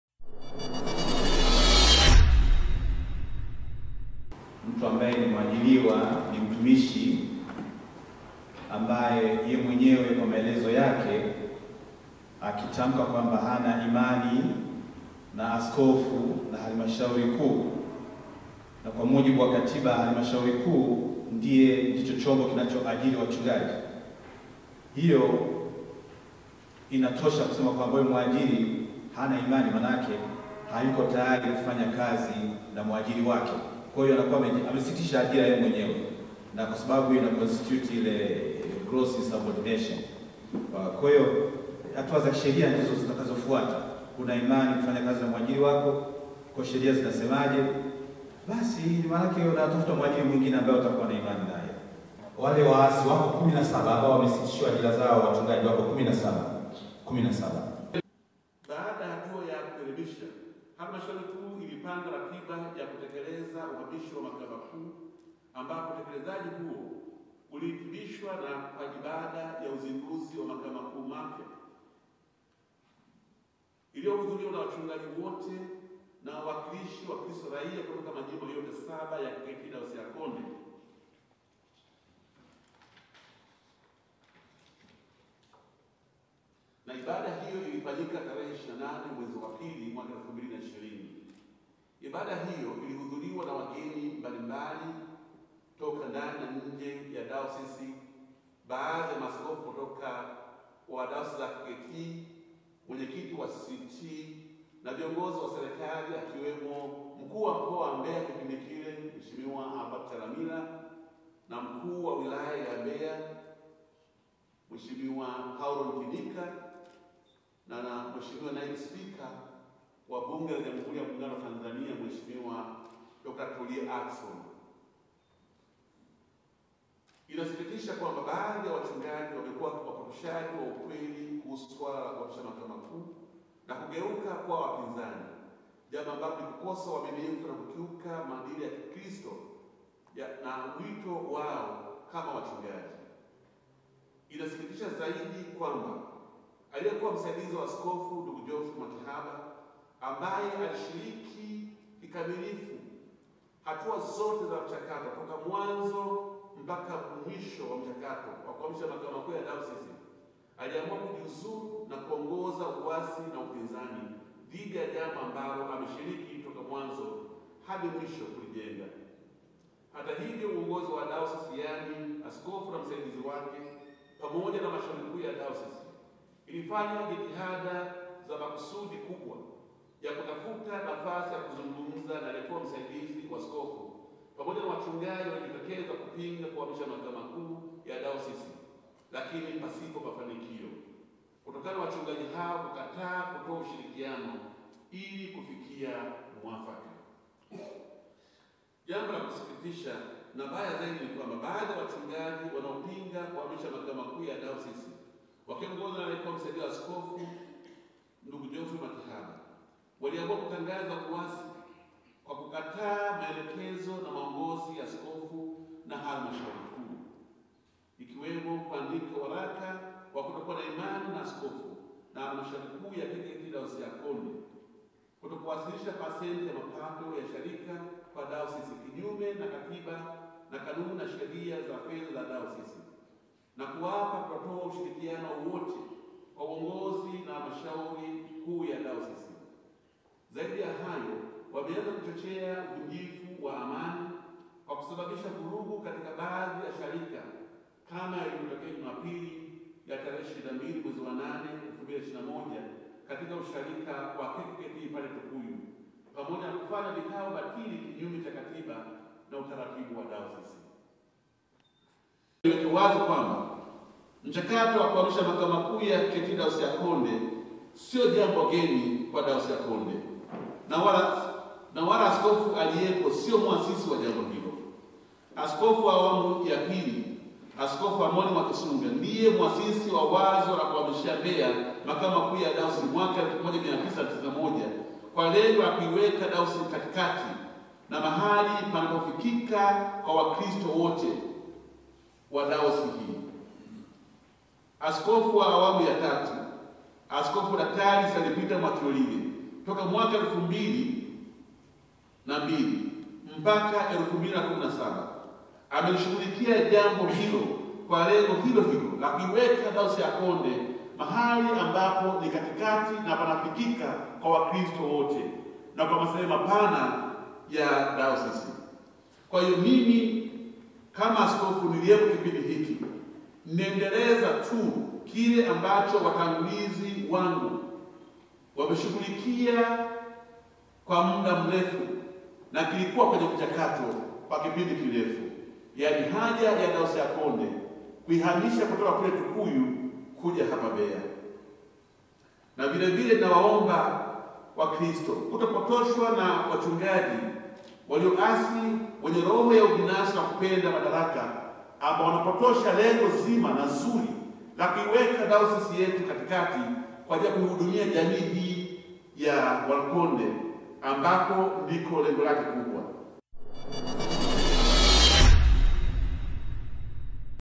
ASKOFU MWAIKALI:WACHUNGAJI 17 WANAOPINGA KUHAMA KWA MAKAO MAKUU YA DAYOSISI NI WAASI
Wachungaji Kumi na Saba wa Kanisa la kanisa la kinjili la kilutheri Tanzania Dayosisi ya Konde wamesimamishwa kazi Kwa kukiuka maadili ya kukiuka maadili ya kikristo na wito wa kichungaji.

Akizungumza na wandishi wa Habari Makao makuu ya Dayosisi ya Konde jijini Mbeya Katibu Mkuu wa KKKT Dayosisi hiyo Mchungaji Ikupilika Mwakisimba amesema wachungaji hao wamekuwa wakipinga kuhamishwa Makao makuu ya Dayosisi kutoka Tukuyu wilayani Rungwe kwenda Jijini Mbeya.